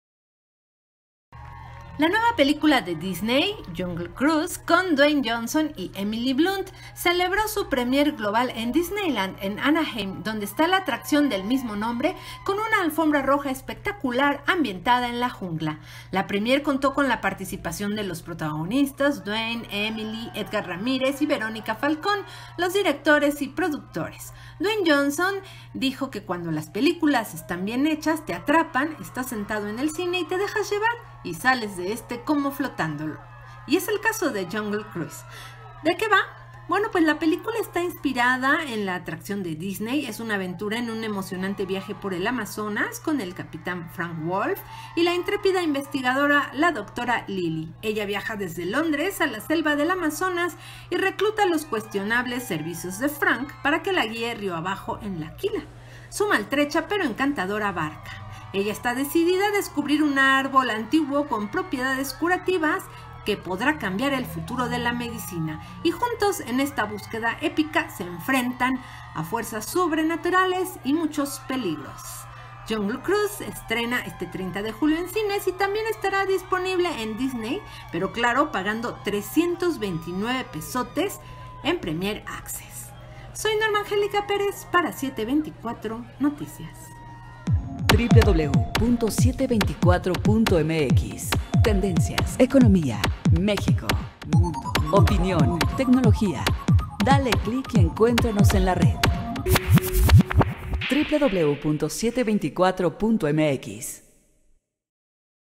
Así celebró Jungle Cruise su premier en Disney
La nueva película de Disney, Jungle Cruise, con Dwayne Johnson y Emily Blunt, celebró su premier global en Disneyland, en California, con una alfombra roja espectacular ambientada en la jungla.